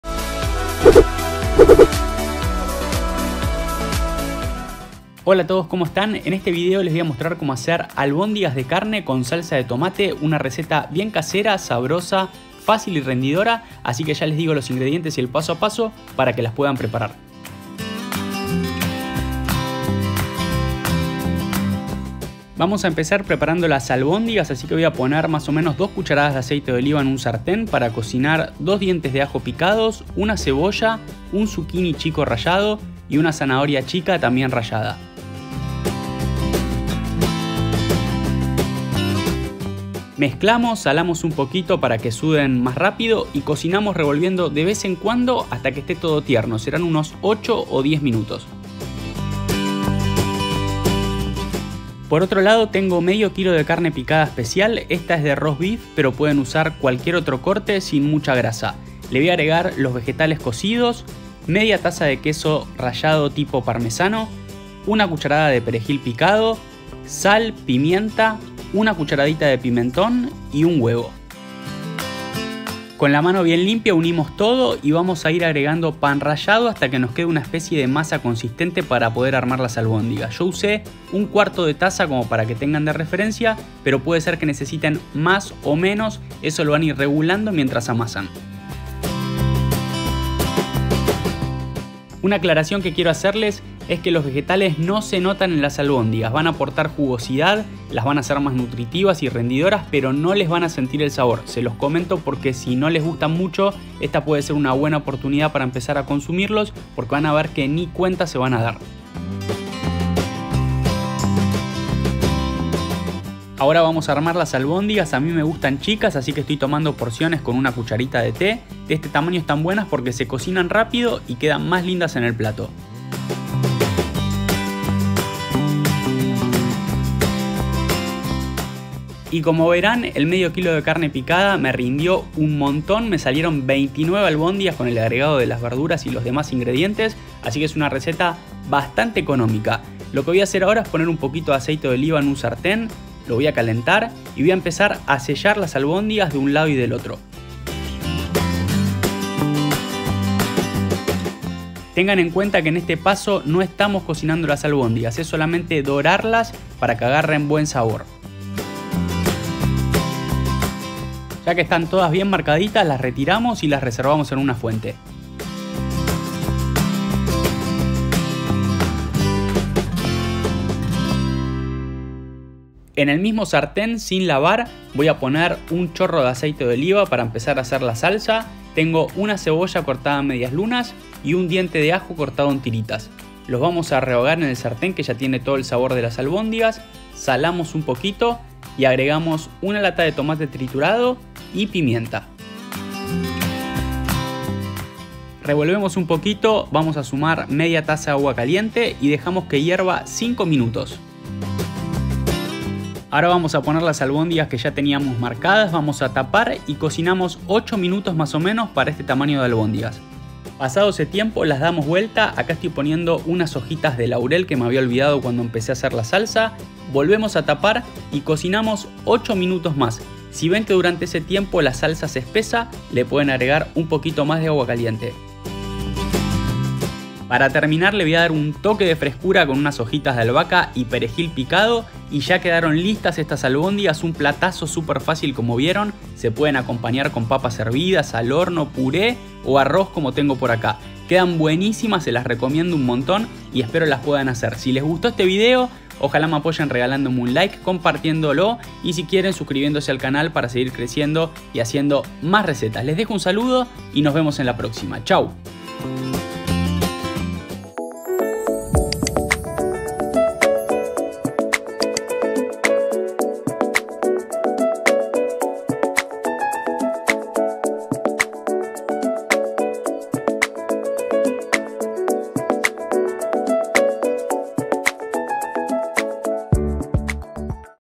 Cómo Hacer Albóndigas de Carne Caseras con Salsa Muy Fáciles, Sabrosas y Rendidoras / MONO 1981
Cómo Hacer Albóndigas de Carne Caseras con Salsa Muy Fáciles, Sabrosas y Rendidoras

Hola! En este video te muestro cómo preparar unas albóndigas de carne picada con salsa de tomate muy fáciles, sabrosas y rendidoras. Ideales para acompañar con puré, papas hervidas, arroz o fideos y disfrutar de un súper plato!

Ingredientes

Albóndigas

1/2 kg de carne picada
2 dientes de ajo
1 cebolla
1 zucchini chico
1 zanahoria chica
Aceite, sal, pimienta
1/2 taza de queso tipo parmesano rallado
1 cda de perejil
1 cdta de pimentón
1 huevo
Cantidad necesaria de pan rallado

1 lata de tomate
1/2 taza de agua
1 cebolla
1 diente de ajo
Aceite, sal, pimienta
2 hojas de laurel
1 taza de agua
Albahaca, a gusto
Perejil, a gusto